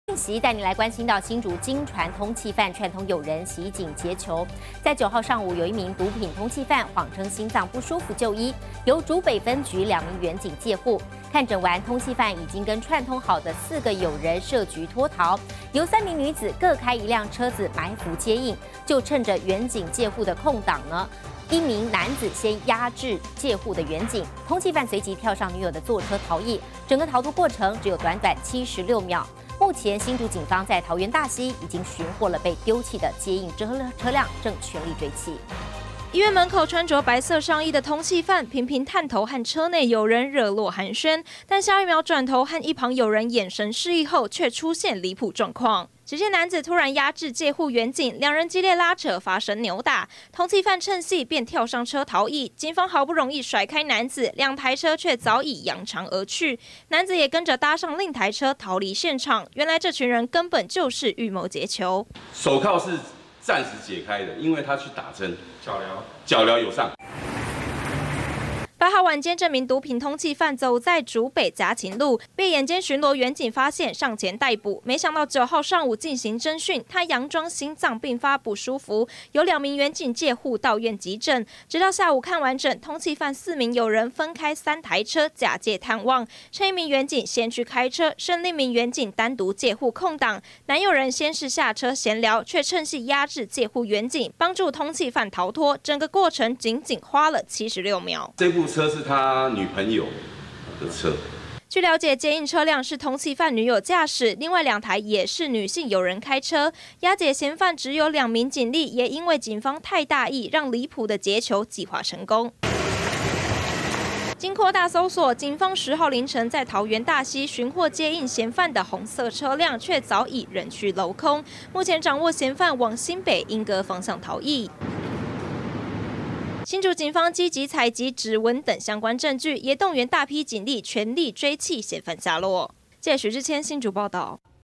預謀劫囚!1警開車1人單獨戒護 男壓制警扭打
新竹驚傳通緝犯串通友人，襲警劫囚。9號上午一名毒品通緝犯謊稱心臟不舒服就醫，由竹北分局兩名員警戒護，看診完，通緝犯和已經串通好的4名友人設局脫逃，由3名女子各開一台車埋伏接應，趁員警單獨戒護空檔，一名男子先壓制戒護員警，通緝犯隨即跳上女友座車逃逸。整個逃脫過程，僅僅花了76秒。目前新竹警方在桃園大溪尋獲被丟棄的接應車輛，也全力追緝嫌犯歸案，警方懷疑整起逃脫案由女友策劃，還找來嫌犯的紅粉知己配合，上演通緝犯大逃脫。